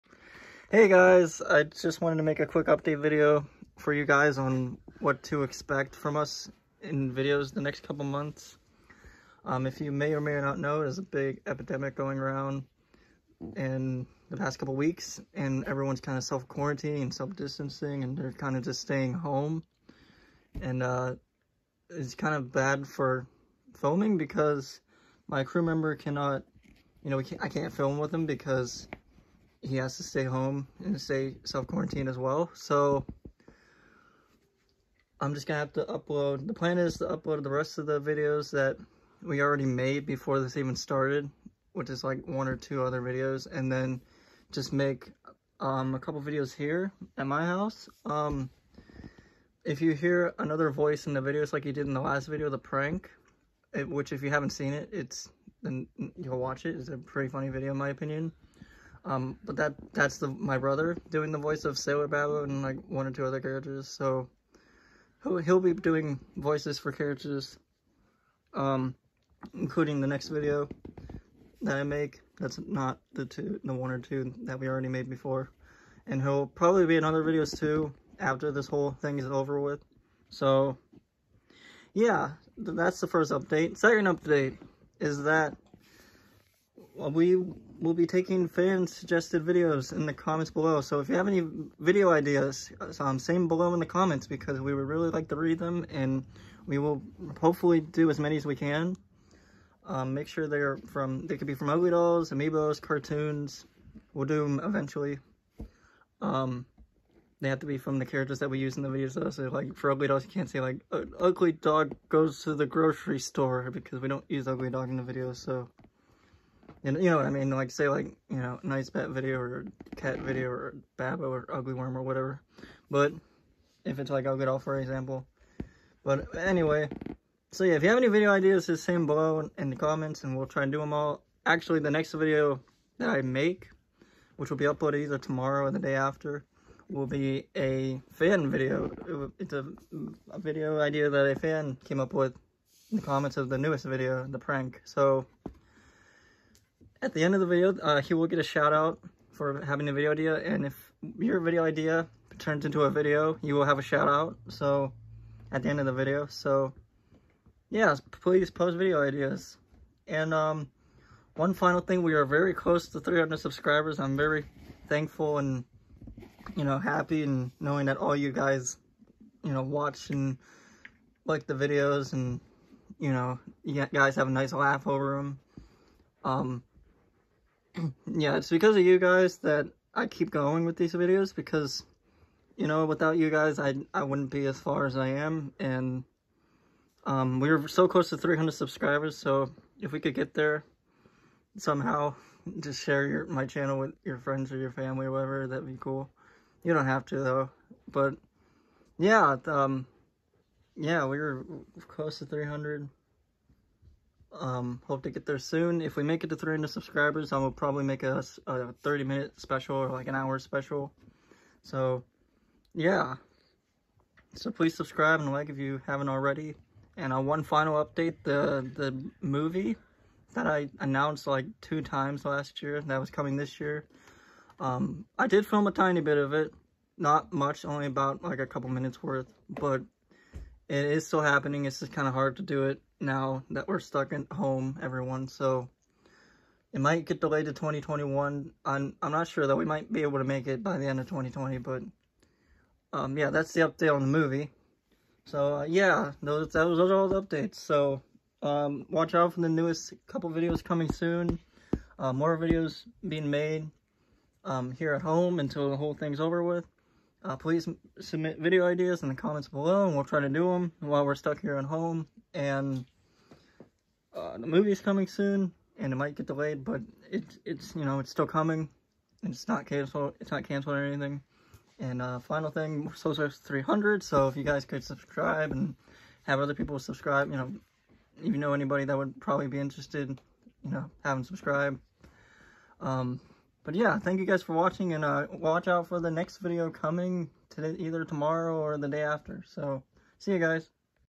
Important update video! Movie news, fan ideas, video schedule, 300 subs, and more!
This is just a quick update video about all the new stuff coming to the channel and our new schedule for videos during the next few months.